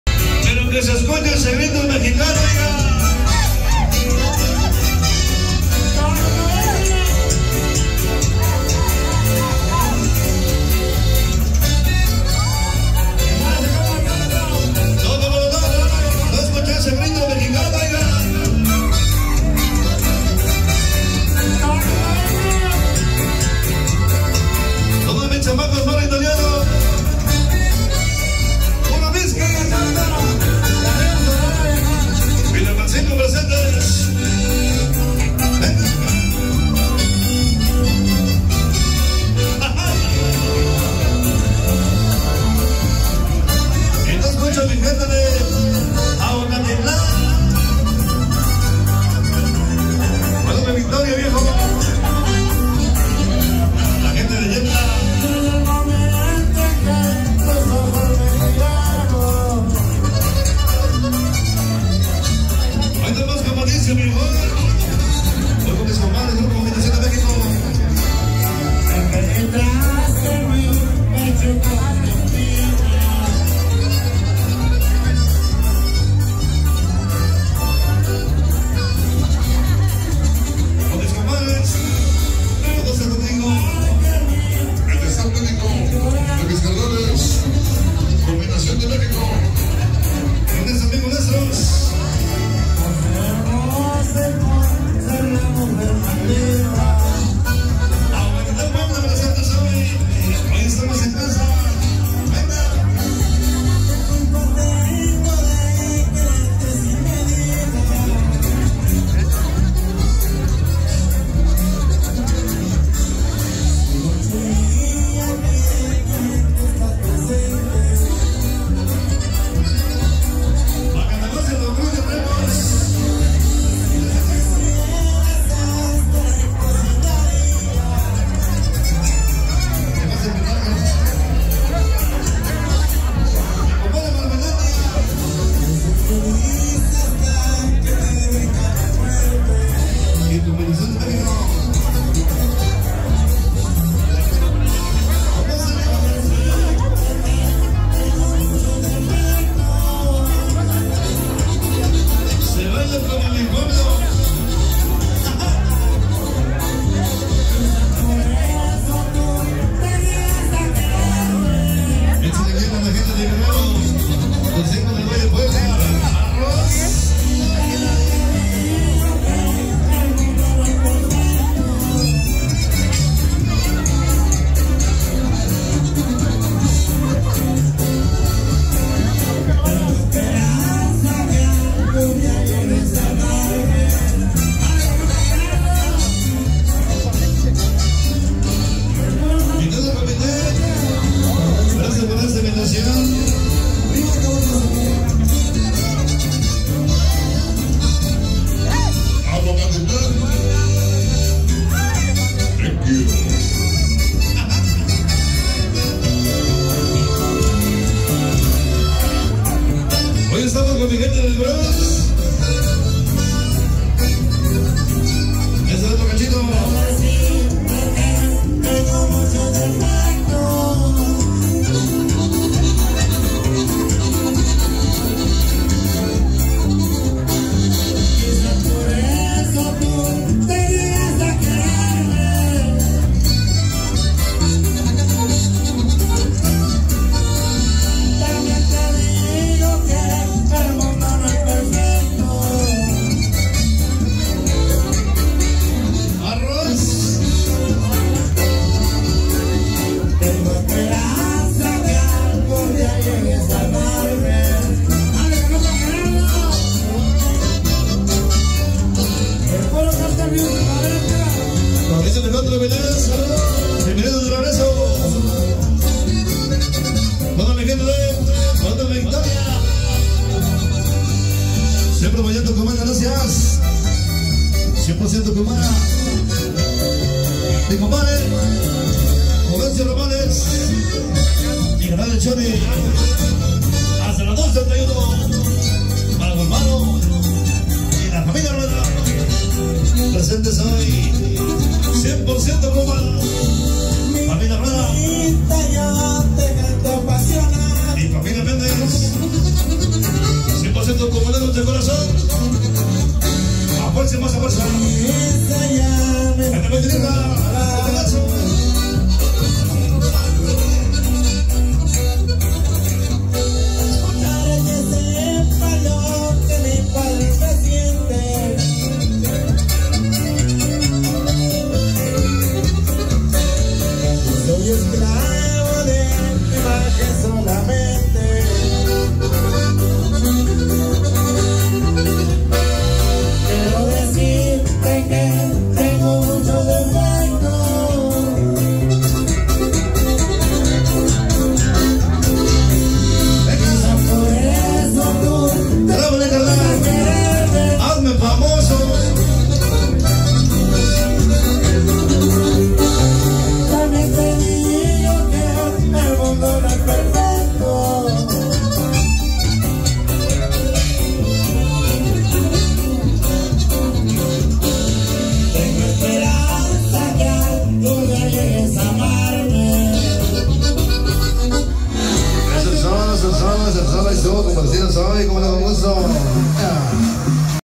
Baile de Aguacatitlan con sonido Kumbala en el Bronx new York 01/13/24
Recaudación de fondos aguacatitlan tulcingo puebla